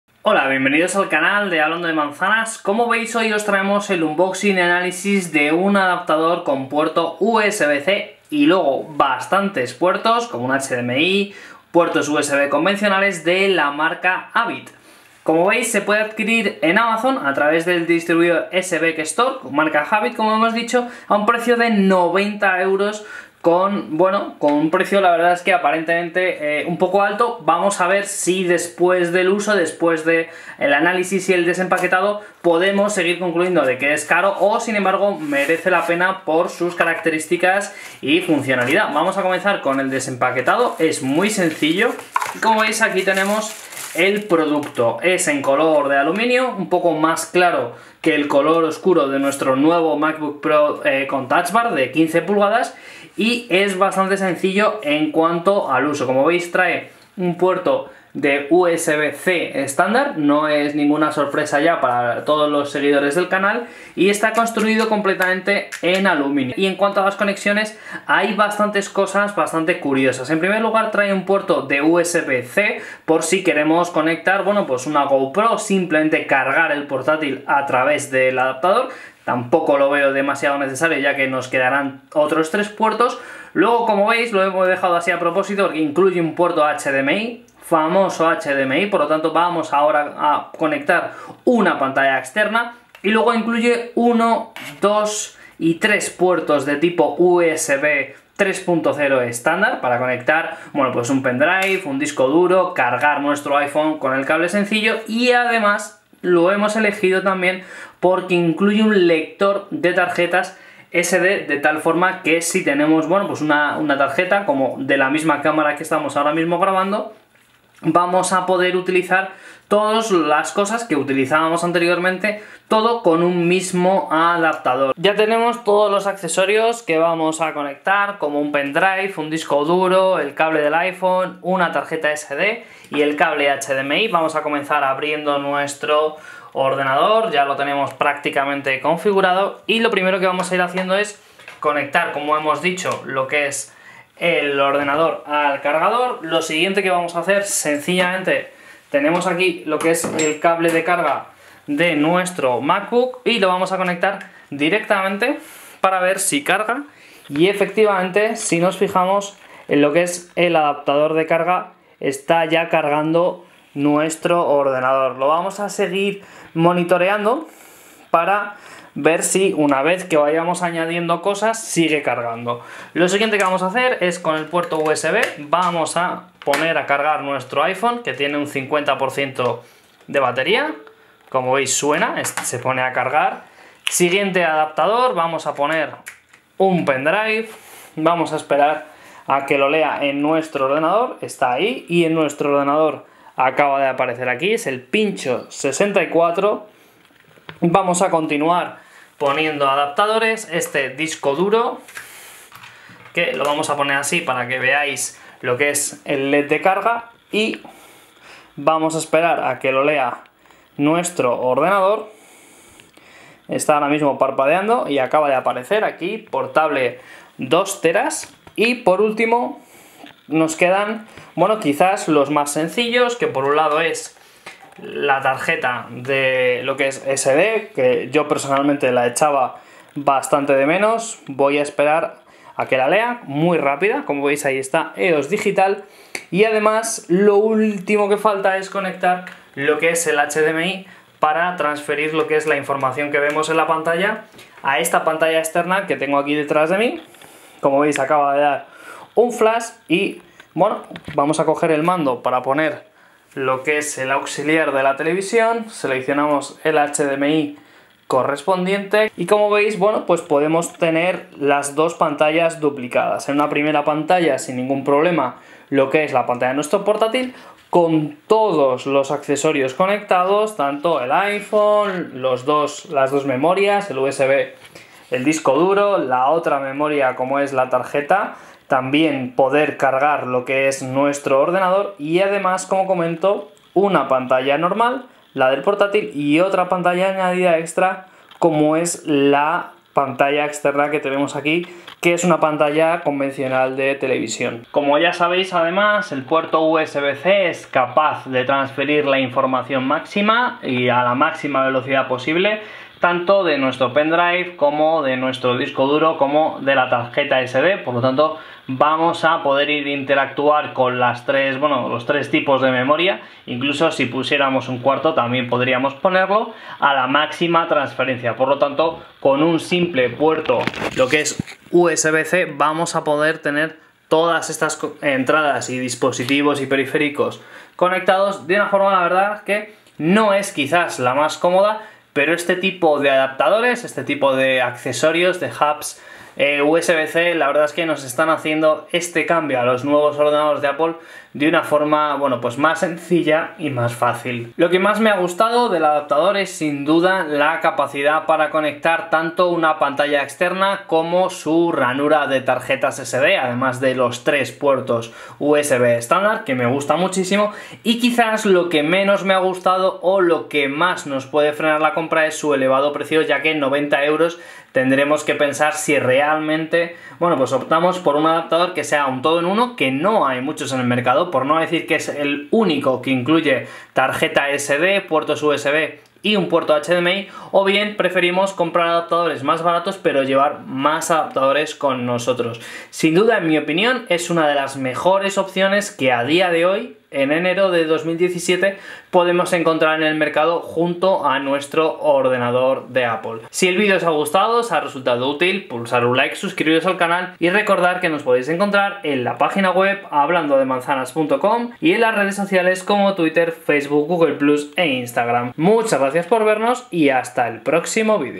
Adaptador con HDMI lector de tarjetas SD y 3x USB de Havit
En este video analizamos un adaptador con conexión HDMI, lector de tarjetas SD y 3x puertos USB 3.0, es de la marca Havit y puede resultar un amigo perfecto con un nuevo MacBook Pro con Touch Bar de Apple.